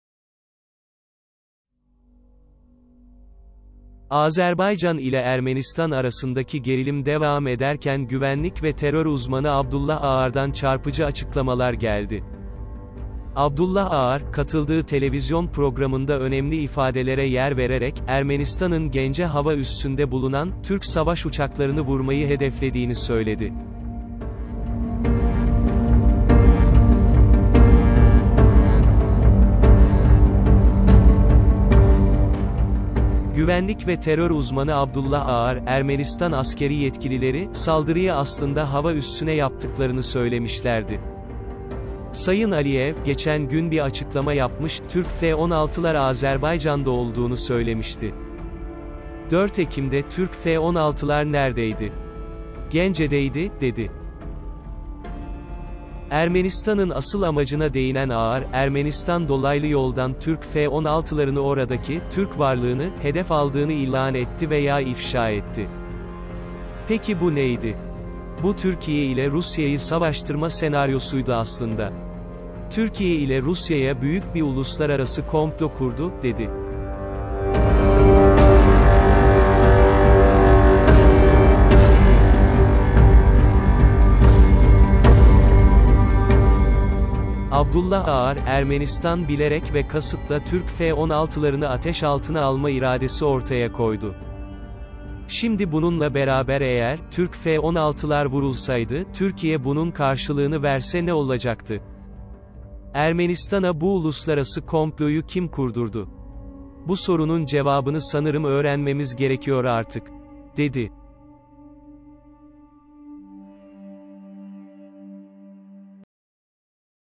Abdullah Ağar planı açıkladı: F-16'LARI VURMAK İÇİN ATEŞLEDİLER SONRA...
Güvenlik ve Terör Uzmanı Abdullah Ağar, katıldığı televizyon programında önemli açıklamalarda bulundu. Ağar yaptığı açıklamada, Ermenistan'ın Gence hava üssünde bulunan "Türk F-16"larını vurmayı hedefliyordu ifadelerini kullandı.